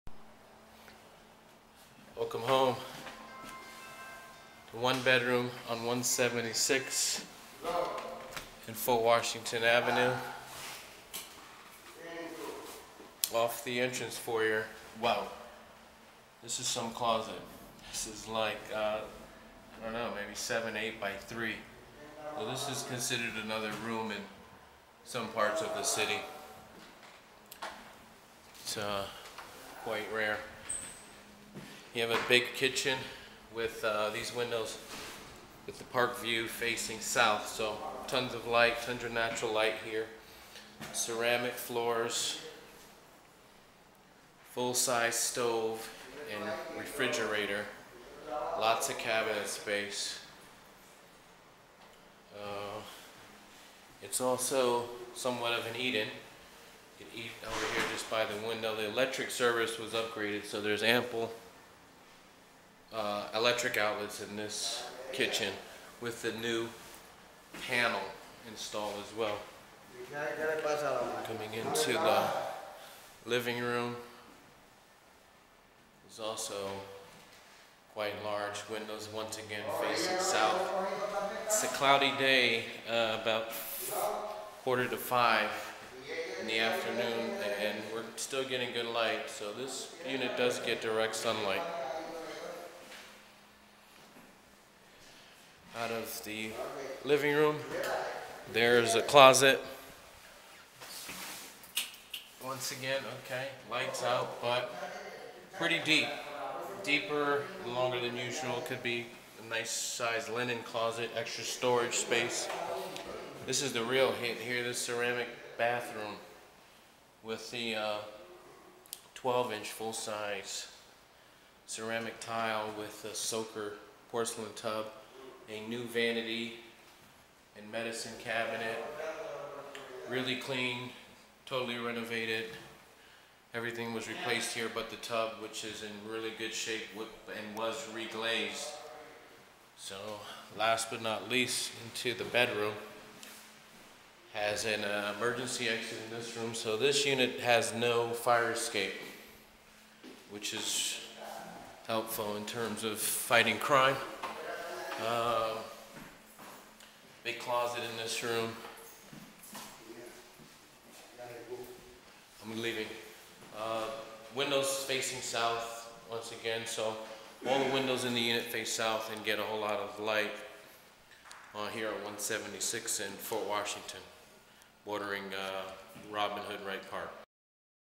Great 1br on 176th and Haven Ave just 5 mins from New York Prebs with great park views
Hudson Heights Rentals, Washington Heights Rentals, Harlem Rentals, Cheap NYC rentals, New York City Rentals